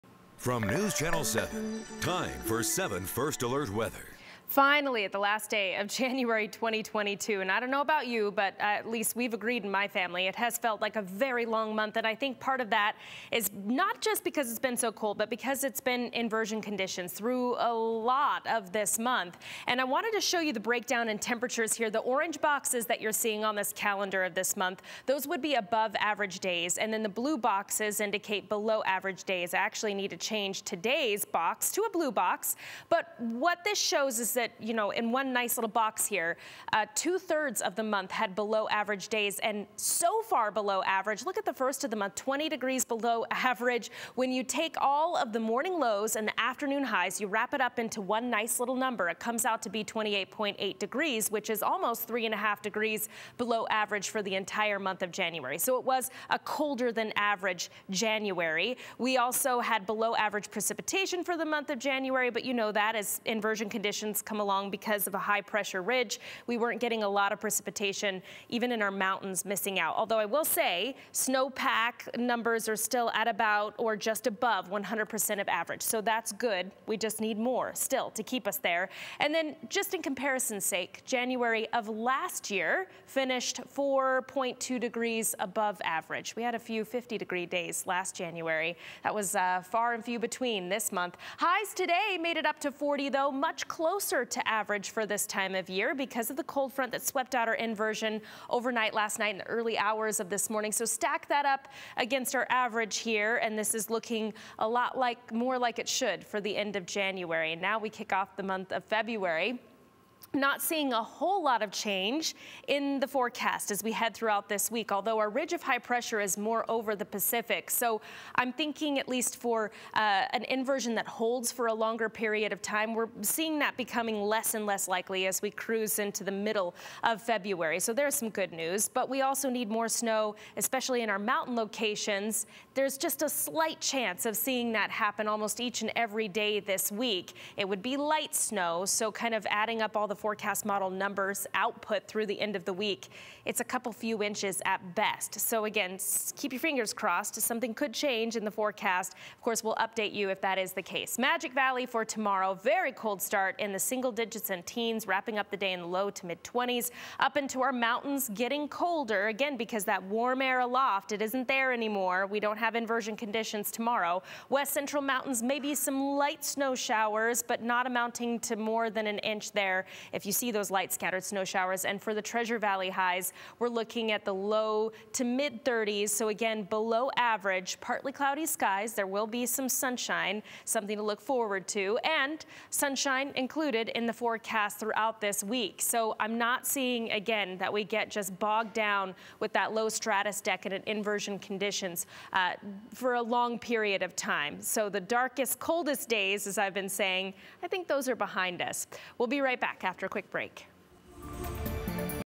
Southwest Idaho evening weather forecast for Jan. 31: Back to colder than normal temperatures
A cold front moved through last night, bringing some light snow showers to some during the very early hours of the morning.  Under a northwesterly flow aloft, there's still enough moisture and instability to see light, scattered snow showers in the west central and central mountains.  Up to an inch of additional accumulation is possible for mountain valleys, with 2-3" in the higher peaks.

Even though it was a cold front that moved through, our valley temperatures have warmed today.  That's because the front was able to sweep out the inversion.  Unfortunately, we get a resurgence of cold air into the region for the rest of the week -- so it's back to below normal temperatures, at least through next weekend.

A dry north, northwesterly flow will dominate this week.  Every once in a while we may see a weak impulse sneak through, which could bright some quick, light snow showers to the region.  This looks more likely for mountain locations, but it's also possible for the valley.  Regardless, it won't last long or amount to too much.  We'll keep you posted every step of the way.

The good news is, there is more sunshine to look forward to this week.  As we move through February, it will become less and less likely for an inversion to form and HOLD over the region for days and days.  So, for anyone who needs to hear it -- most of the cloudiest and coldest days are behind us.

Groundhog Day is on Wednesday, and local groundhogs will most likely see their shadows, implying six more weeks of winter for southwest Idaho.  But, who trusts a rodent?!